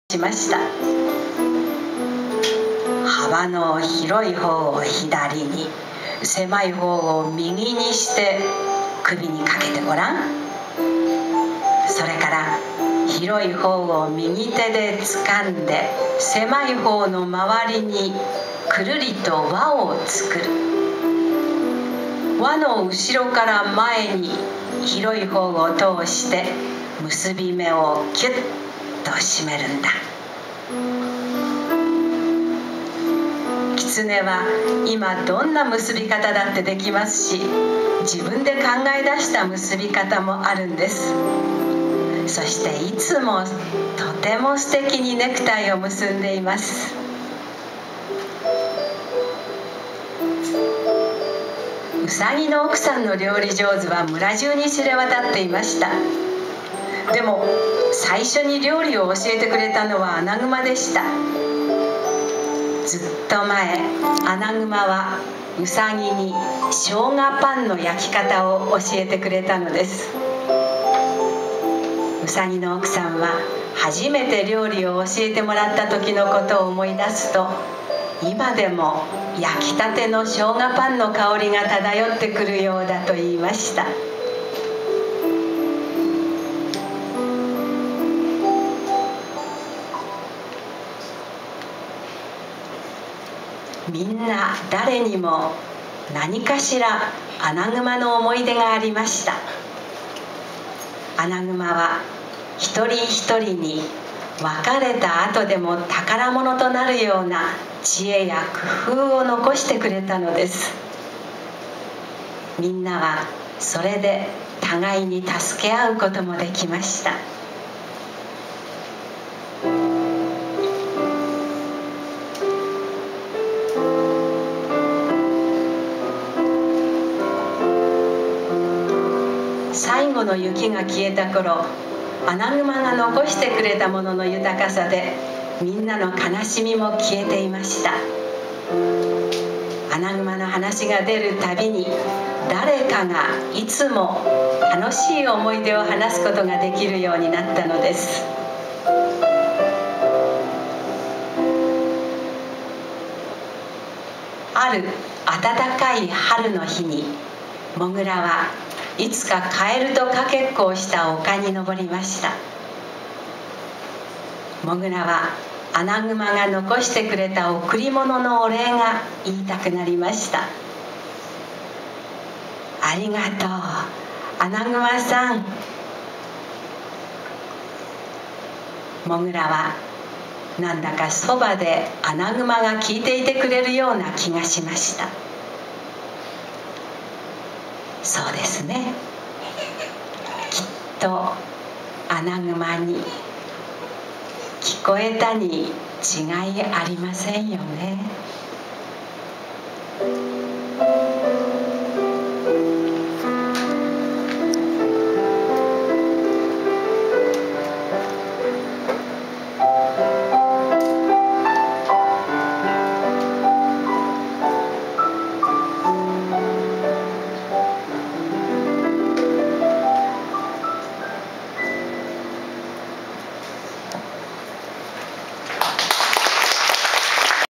第３９回かいろすコンサート＠鹿児島ラーメンみよし家♪３／８、'１２
２０１２年３月８日（木）　第３９回かいろすコンサートが、霧島市の、鹿児島ラーメン　みよし家　牧之原店でありました♪　『わすれられない　おくりもの』スーザンバーレイ作・絵　澄本禎子さん朗読　その③　鹿児島ラーメン　みよし家